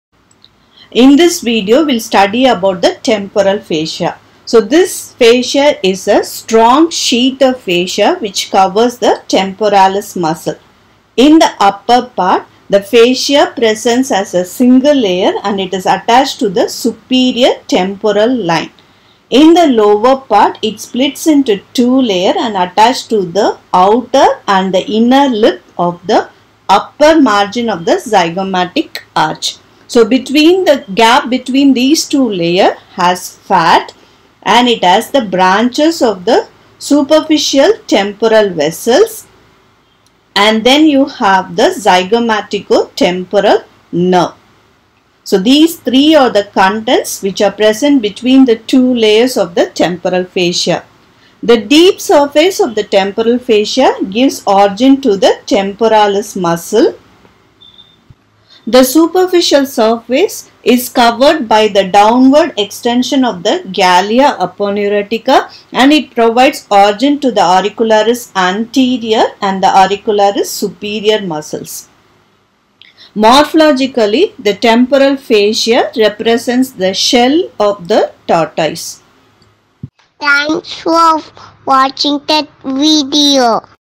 Temporal Fascia | Content Of Temporal Fossa | Attachments | Morphological Importance
This video is about the temporal fascia, Its attachments, structures present between the two layers of temporal fascia in detail.